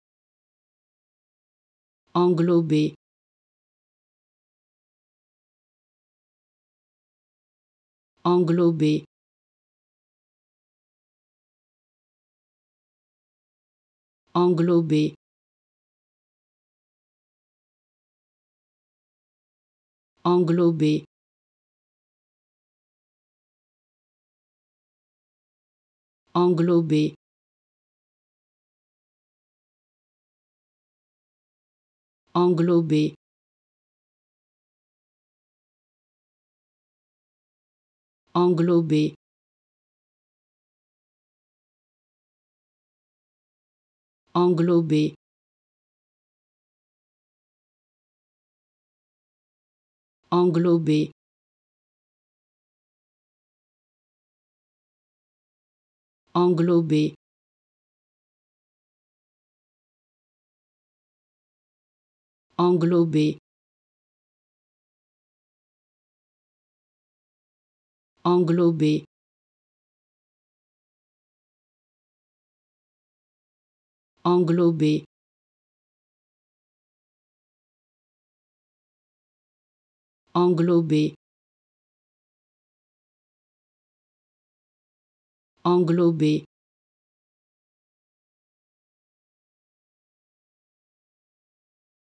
How to Pronounce ''Englober'' (Encompass) Correctly in French
Learn how to say and properly pronounce ''Englober'' in French with this free pronunciation tutorial.